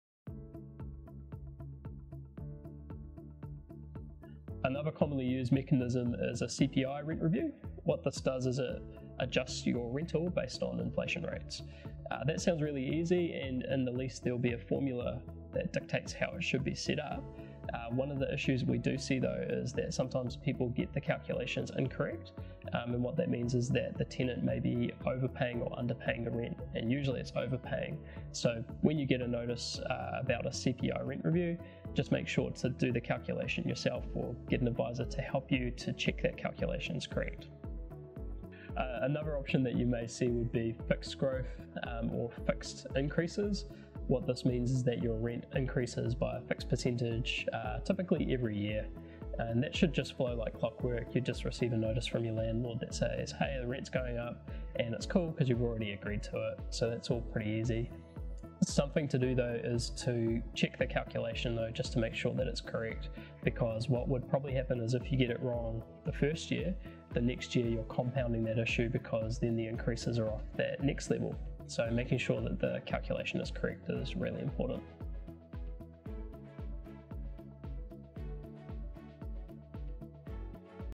Rent reviews: How CPI and fixed rent reviews work - Episode 6
What is a CPI? What does a fixed rent review entail?

In brief, rent reviews are the method by which your rent can change during the term of your lease. A variety of methods exist. Market rent reviews involve the rent being reset to the prevailing market limit. Fixed growth involves the rent growing by a pre-agreed rate (such as 3 percent).

Next in the series: Episode 6 -  How CPI and fixed rent reviews work.